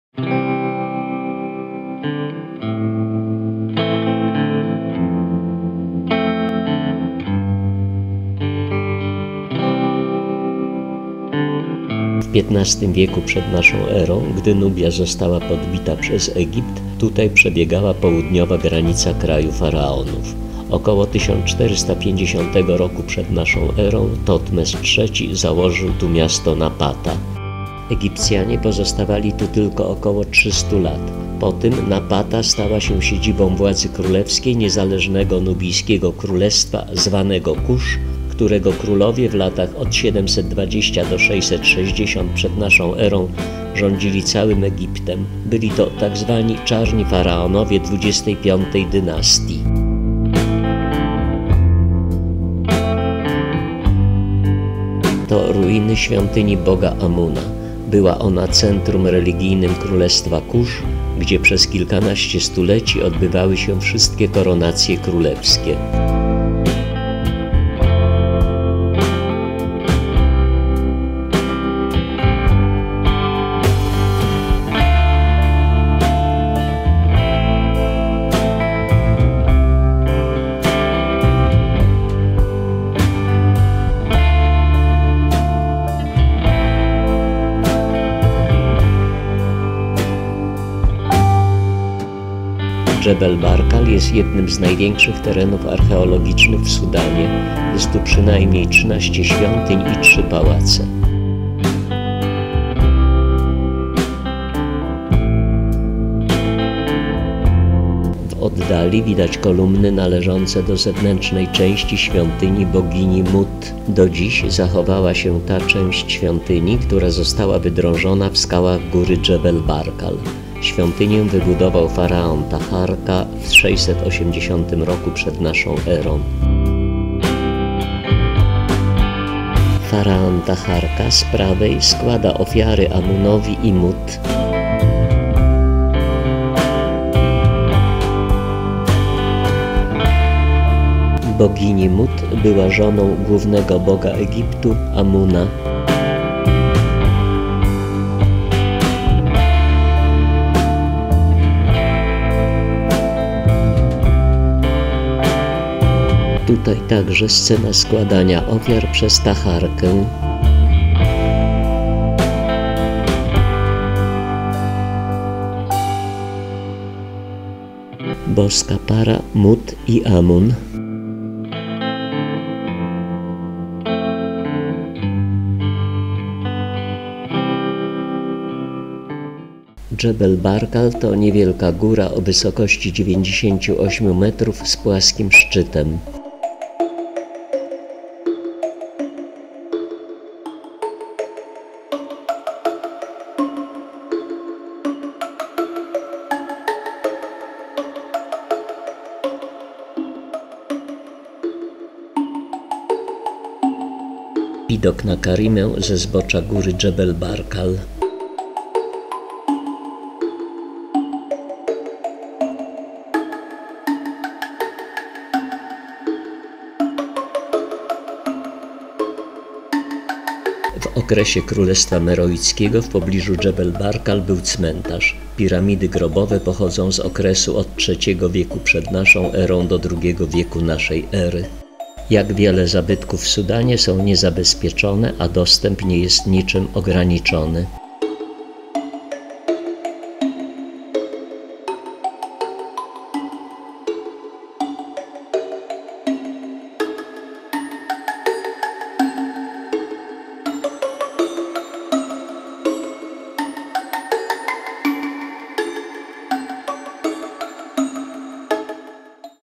Sudan - Jebel Barkal
Jebel Barkal to niewielka góra o wysokości 98 m i płaskim szczycie. W jej sąsiedztwie jest jeden z największych terenów archeologicznych w Sudanie, na którym znajduje się przynajmniej 13 świątyń i 3 pałace. Około 1450 roku przed naszą erą faraon Totmes III założył tu miasto Napata. Po 300 latach egipskiego panowania Napata stała się siedzibą władzy niezależnego nubijskiego królestwa Kusz. Są tu ruiny świątyni boga Amona, w której przez kilka stuleci odbywały się wszystkie koronacje królewskie. Zachowała się także część wydrążonej w skale świątyni bogini Mut, którą wybudował faraon Taharka z XXV dynastii tzw. czarnych faraonów.

Na naszym kanale Filmowe relacje z wycieczek prezentujemy pełną relację z wycieczki do Sudanu.

Zapraszamy i życzymy przyjemnych wrażeń w trakcie oglądania.